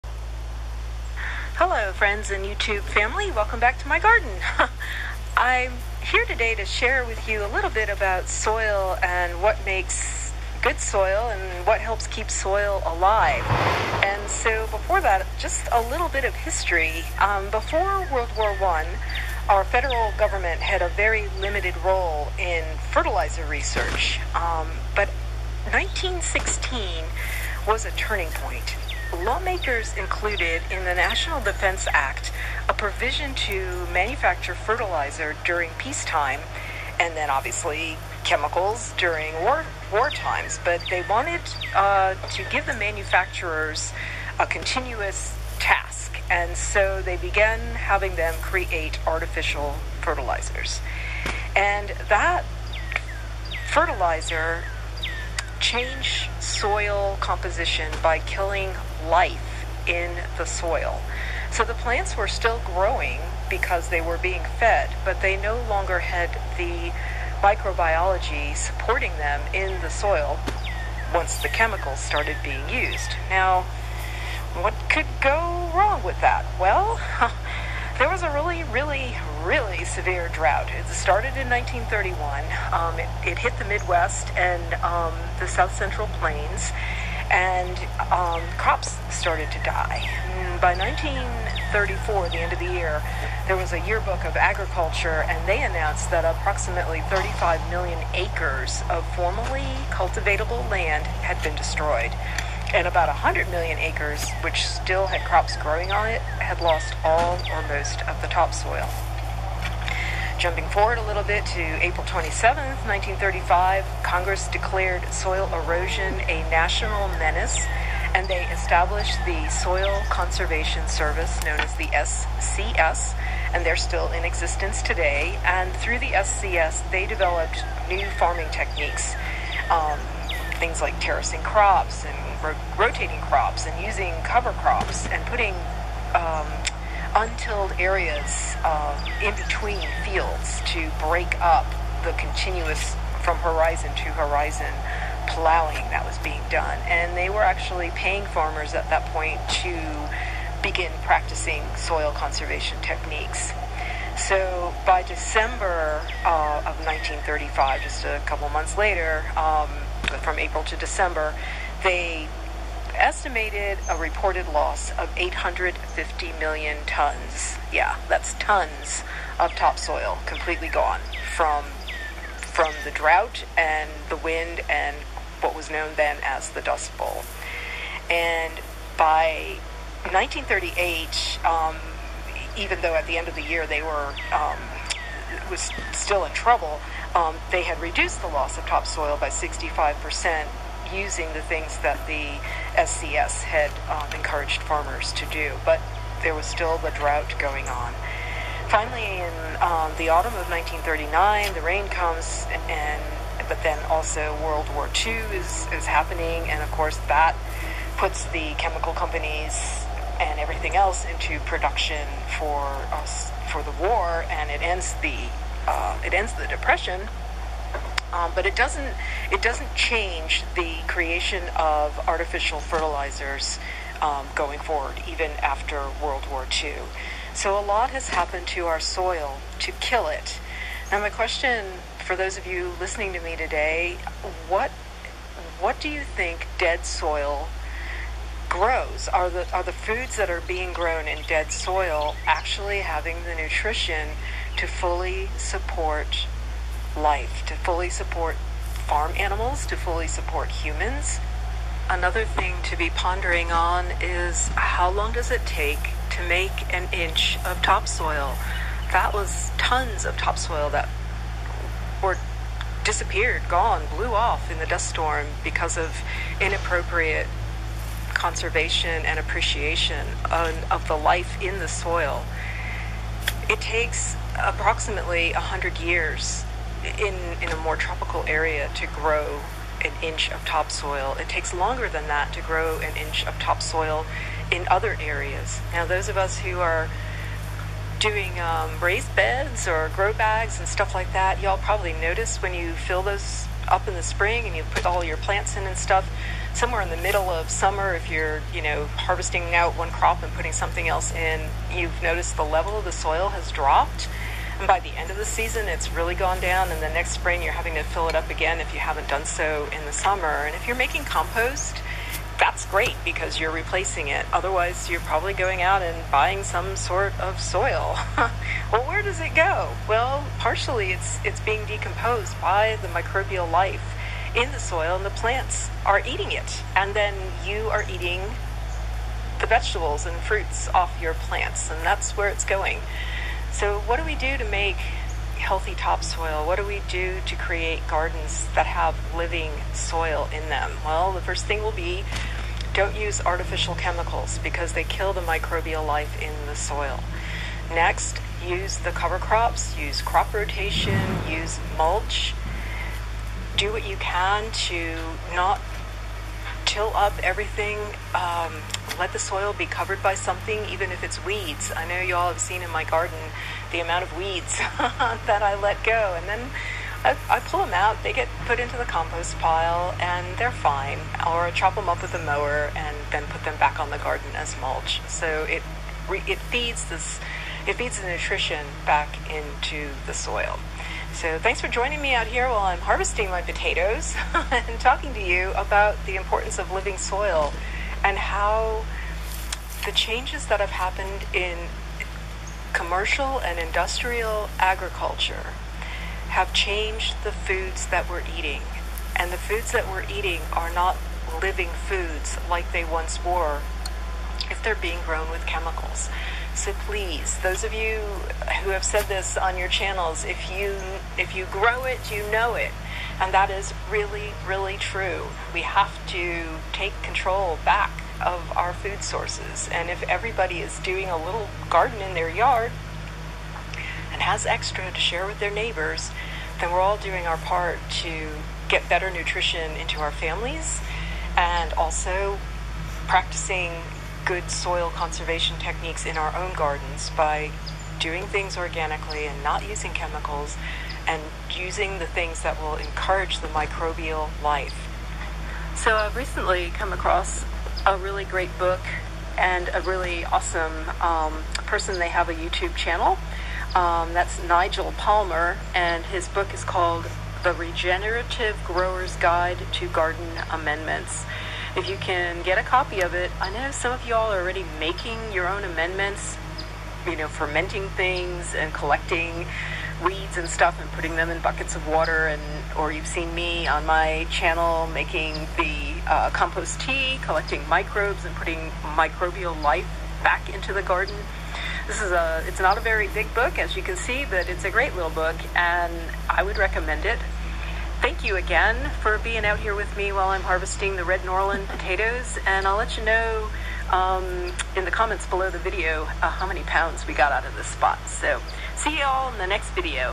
Red Norland Potatoes and Living Soil
Digging Potatoes July 5th. This area is 2 square yards (6ft X 3 ft) and I harvested 12 pounds of Red Norland. [Early Potatoes can yield 6 to 8 pounds per square yard]  Mr Lincoln's Potatoes are still growing.  We all can grow something; those who do know the amazing flavor and quality from homegrown food.  It feels important to discuss some "behind the scenes history" of why chemical agriculture happened, along with what folks can do to maintain and increase microbial and insect life.
 For viewers wondering if my straw mulch kept my Potato plants safe from Potato Beetles... it did... Zero Potato Beetles... yay straw!  When the soil is alive, less pest issues.

Thanks 🙏🏼❣️GG for your phrase " When you grow it, you know it.”

SCS is now called "Natural Resources Conservation Service" NRCS
Please check out Nigel Palmer on YT for more info on his techniques.

🌟The content of this video IS available for folks to copy if interested.

Apologies for the background noise, road maintenance!